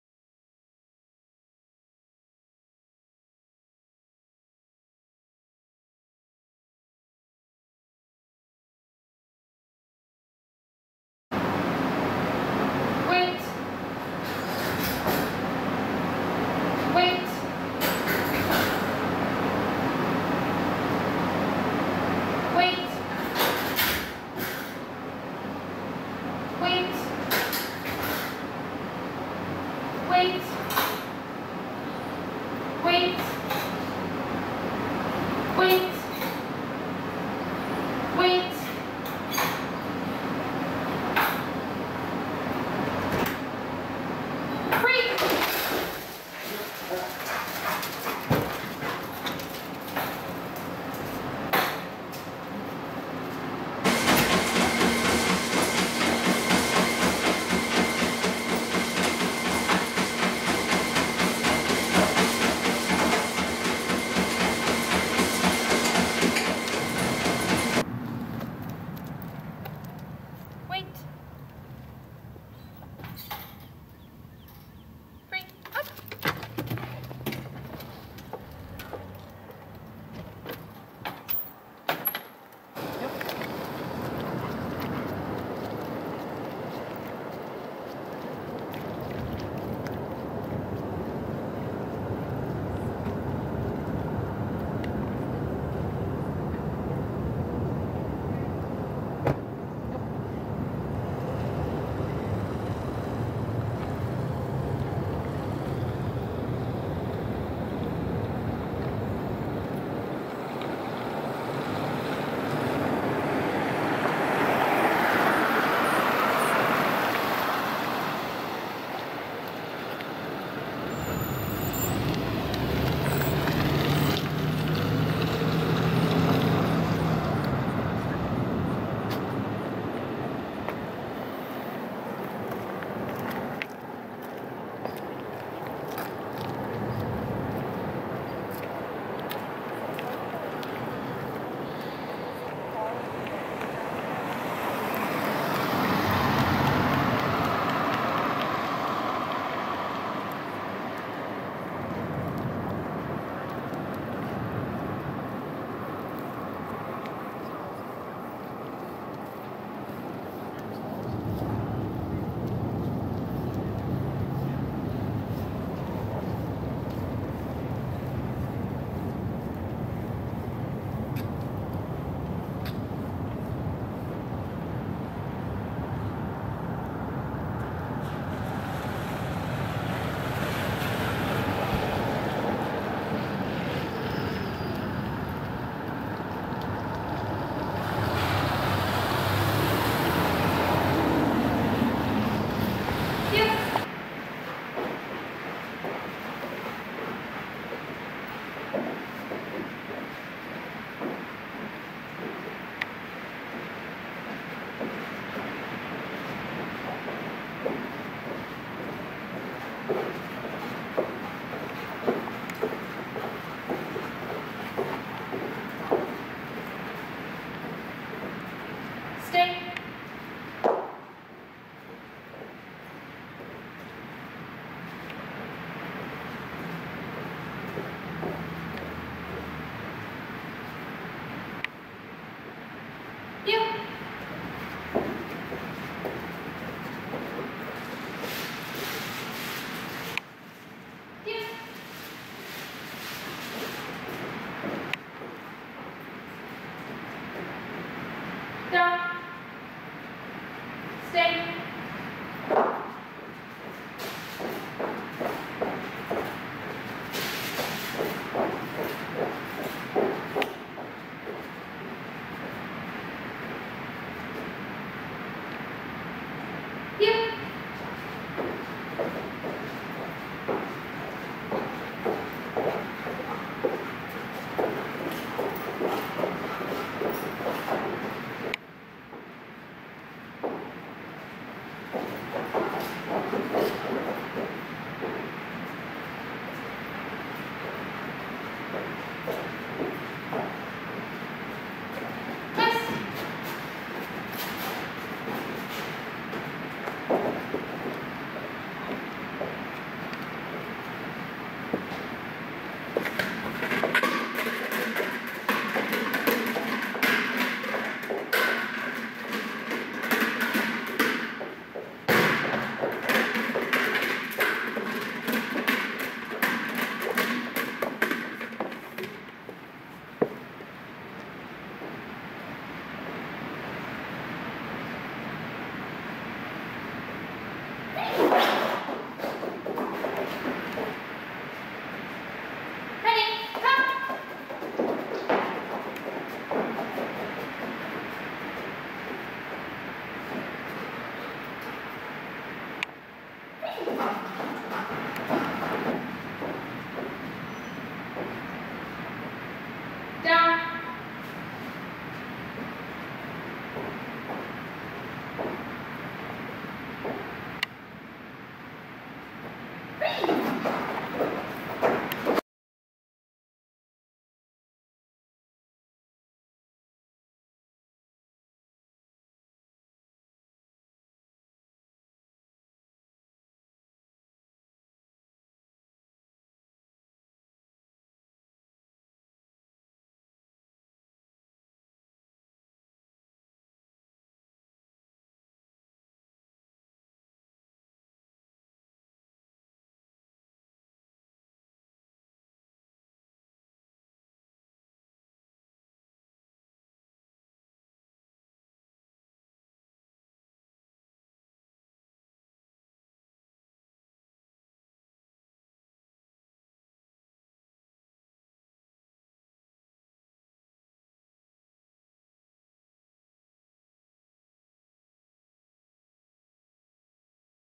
Penny (Labrador Retriever) Boot Camp Dog Training Demonstration
Penny (Labrador Retriever) graduated from the dog training boot camp at Neuman K-9 Academy. This program included obedience commands to sit, stay, heel or walk on a loose leash, come when called, proper etiquette, no jumping up, meeting and greeting people under control, and running on a treadmill.

Our dog training camp provides programs for the Labrador Retriever such as boot camp, obedience training, and puppy camp.